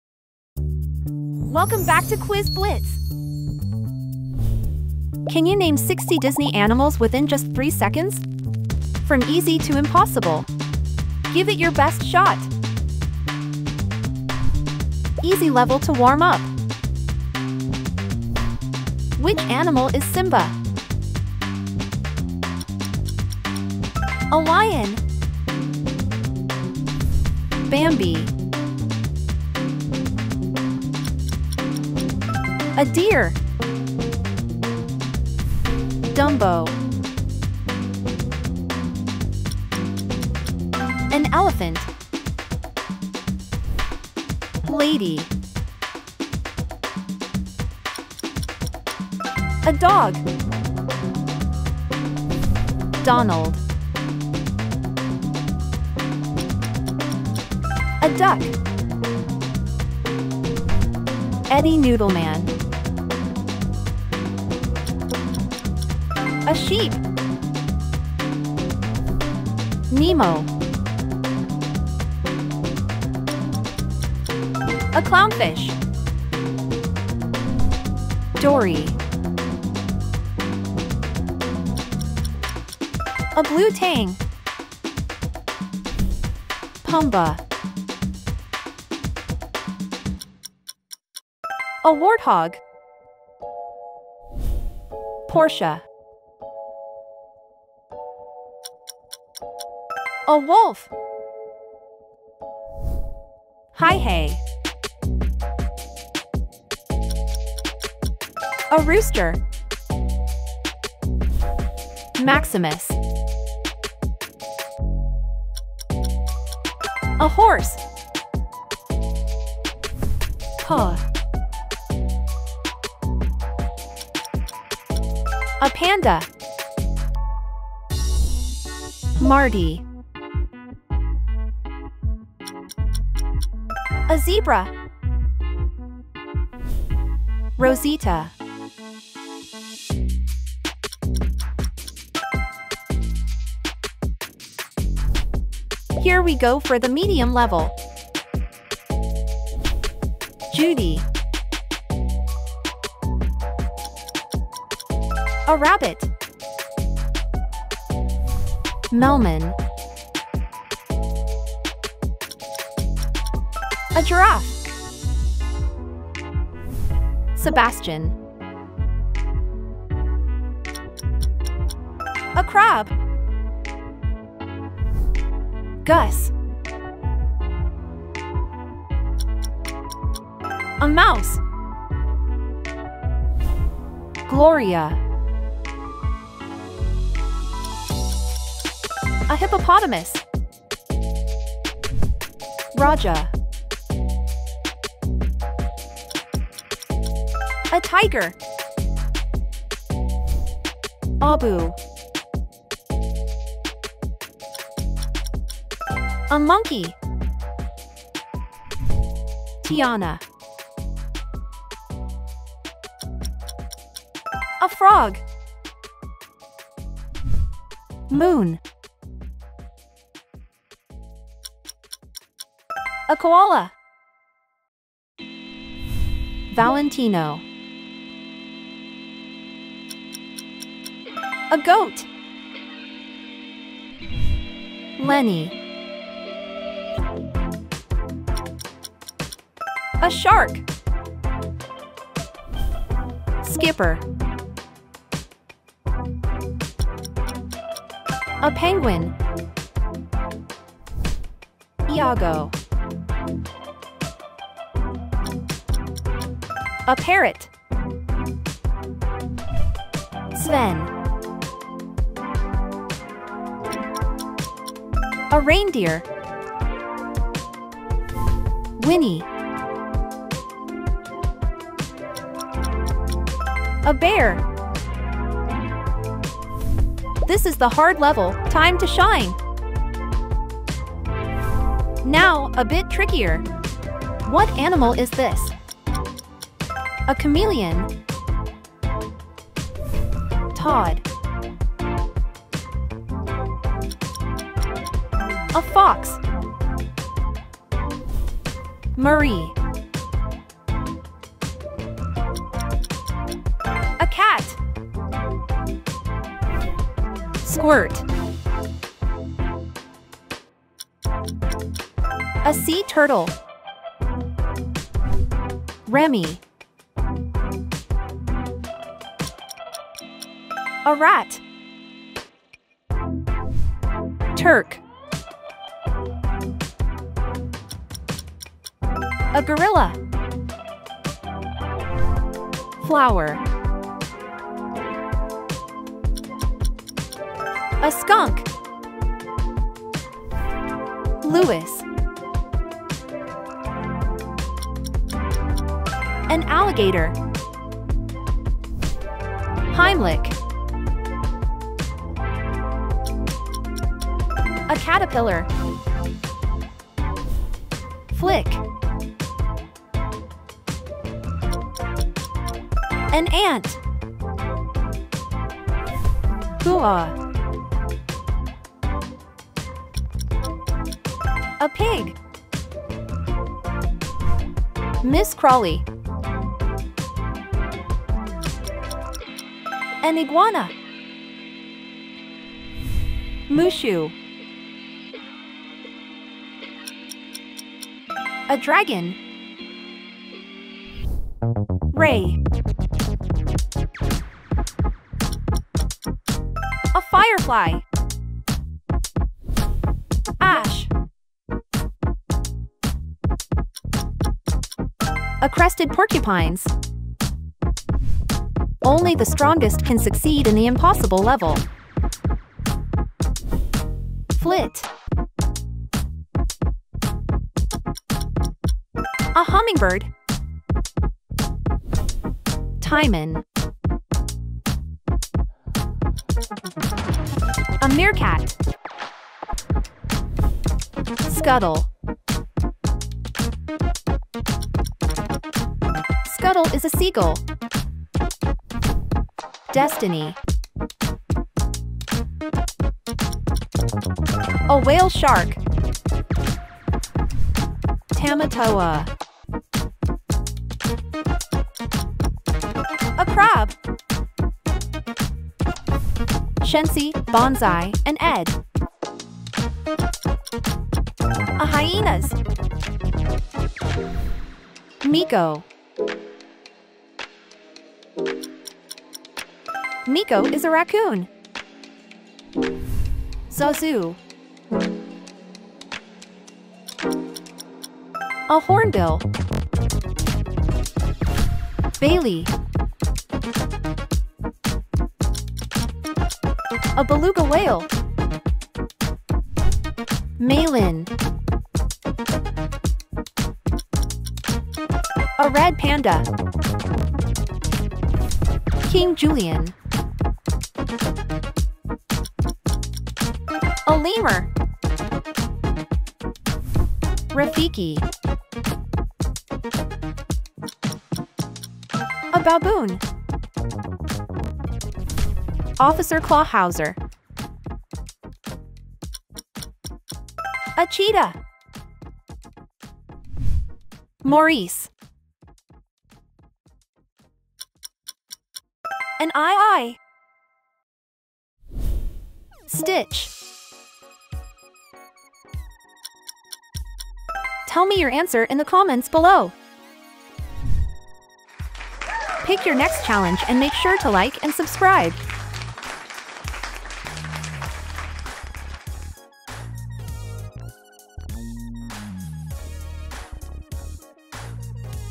Guess 60 Animals Disney in 3 Seconds | Easy, Medium, Hard, Impossible
Can you guess 60 Animals Disney in just 3 seconds? Test your knowledge with this fun and challenging quiz! From easy to impossible, see if you can identify all the Disney animals in record time. Get ready to put your Disney knowledge to the test and see how many animals you can guess correctly. Challenge yourself and your friends to see who can guess the most animals. Let's see if you can guess the animal in this fast-paced quiz!